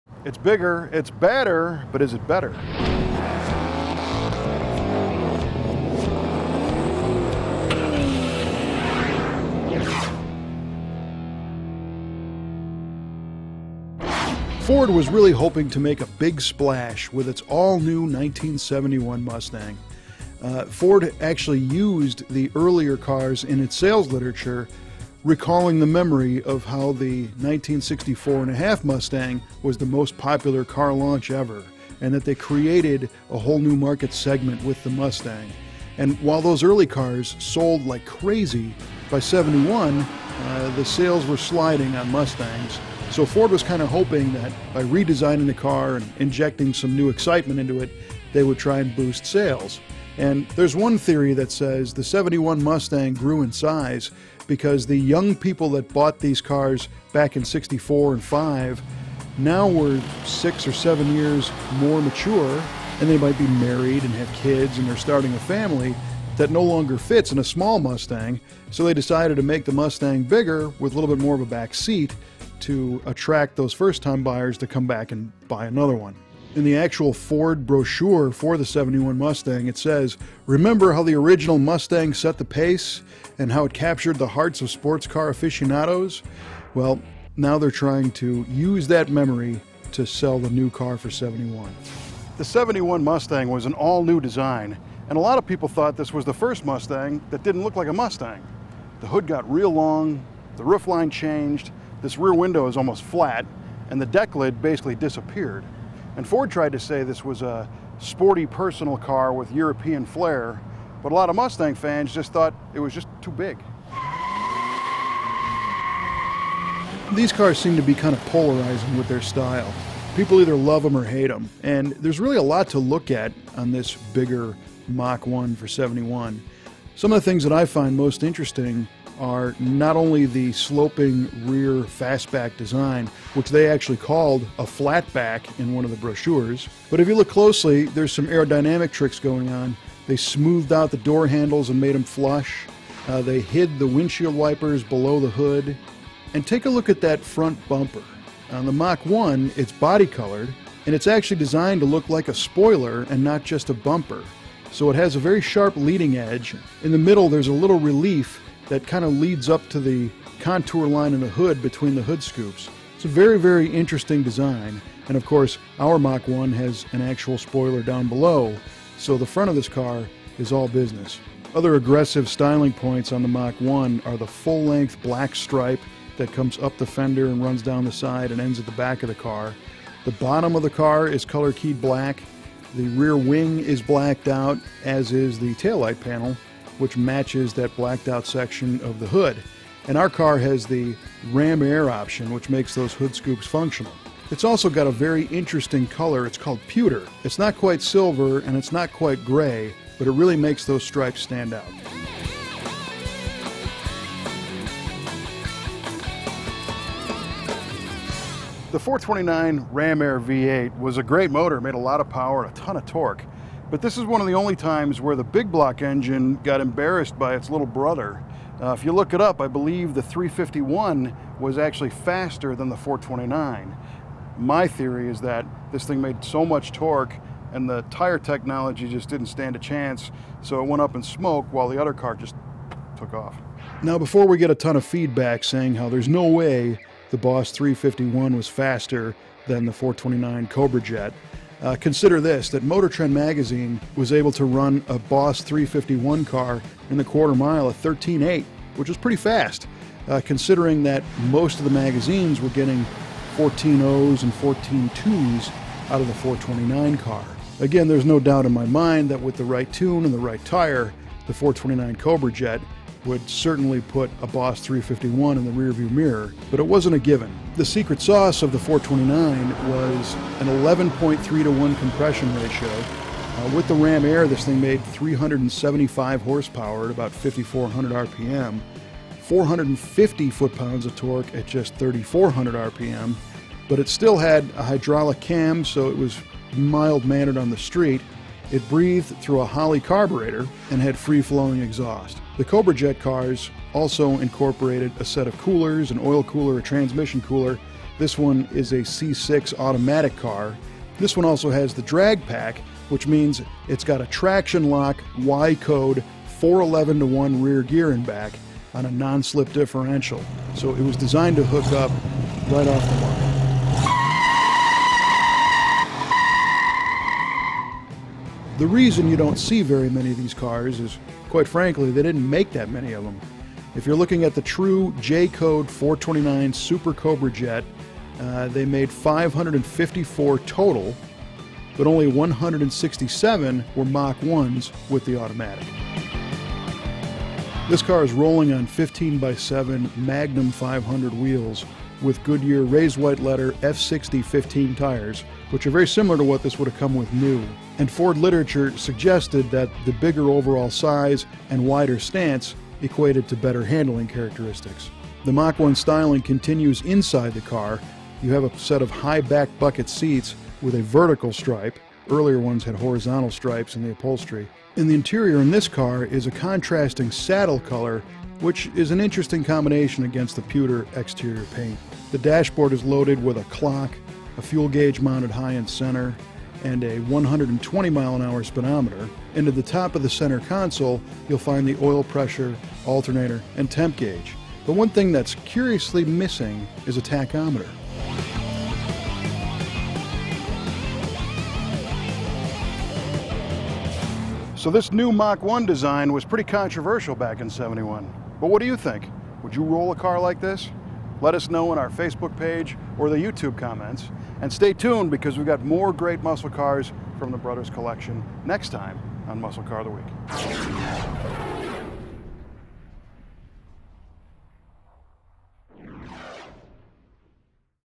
1971 Ford Mustang Mach 1 429 Video Muscle Car Of The Week Episode #125
1971 Ford Mustang Mach 1 429 Video Muscle Car Of The Week Episode #125

1971 showed us and all new Mustang design, and this time, we featuring a full-tilt 429 Super Cobra Jet Mach 1!  What do you think of this generation of Mustang? From the Brothers collection.